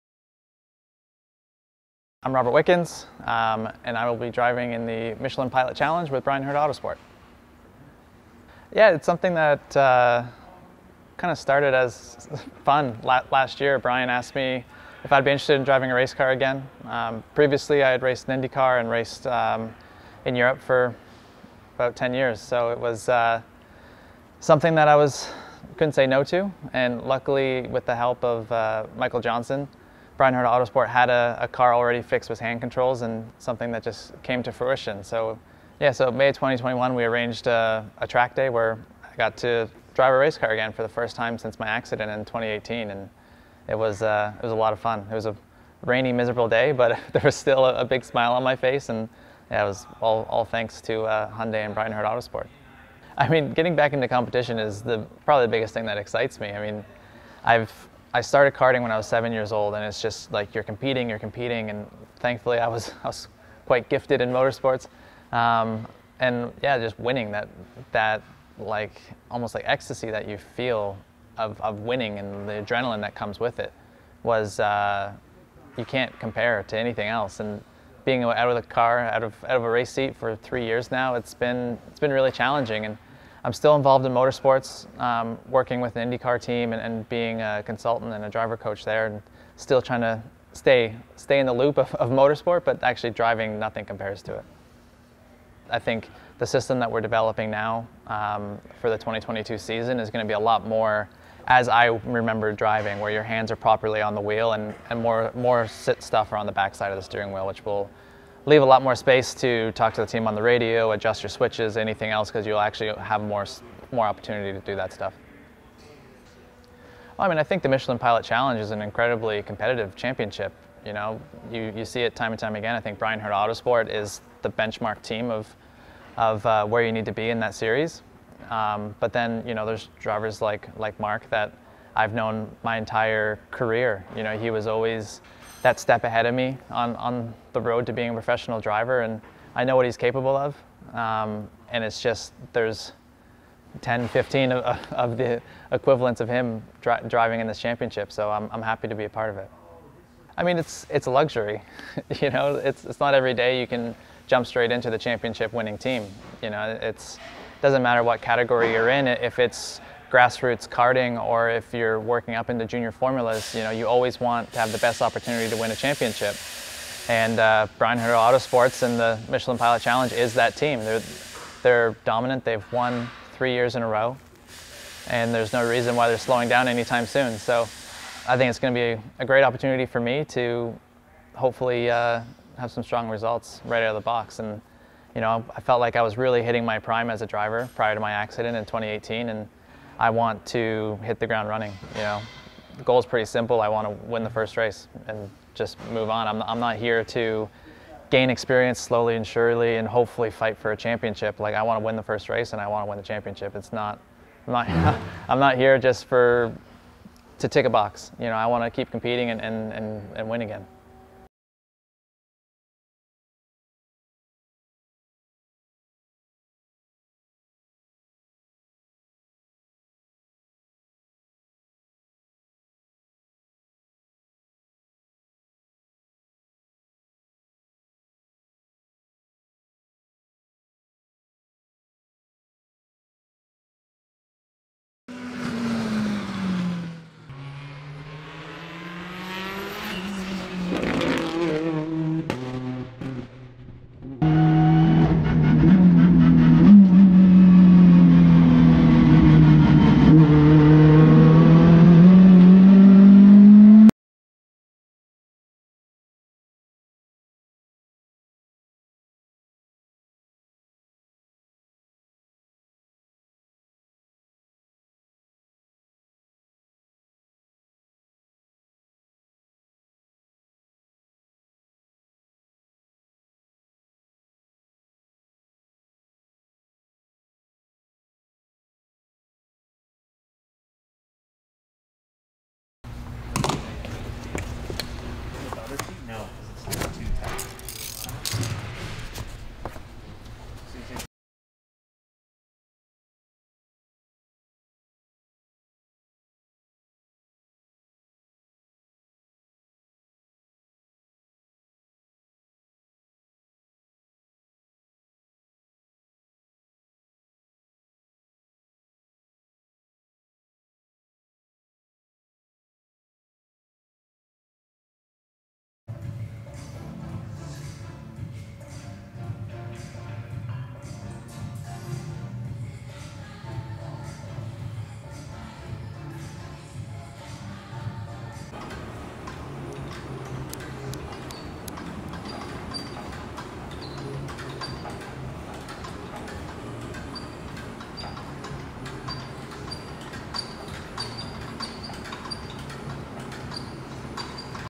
Robert Wickens Will Return To Racing With Bryan Herta Autosport
Indycar star Robert Wickens will return to competition during a four-hour race at Daytona International Speedway behind the wheel of a Hyundai Elantra N TCR. The four-hour endurance race held at the Daytona International Speedway will be his first since he suffered a spinal injury in 2018.